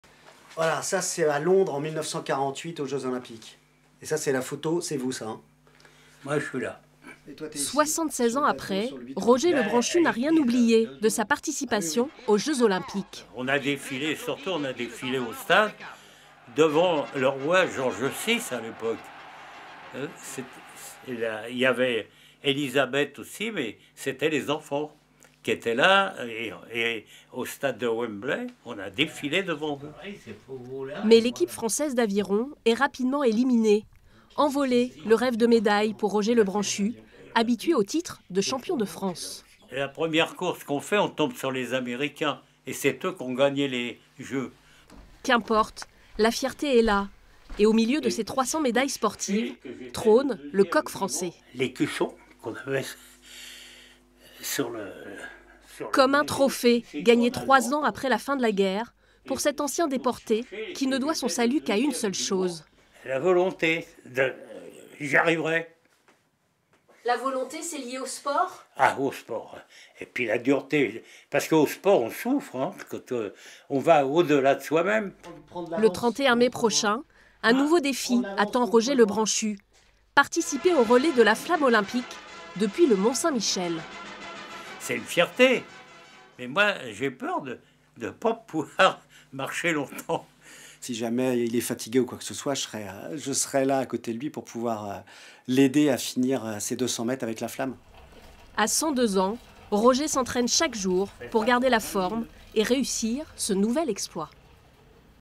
Roger Lebranchu, doyen des porteurs de la flamme olympique
À bientôt 102 ans, Roger Lebranchu sera le doyen des porteurs de la flamme olympique qu'il portera fièrement le 31 mai prochain au Mont Saint-Michel. Un beau cadeau pour cet ancien athlète multimédaillé qui a notamment participé aux Jo de 1948 à Londres.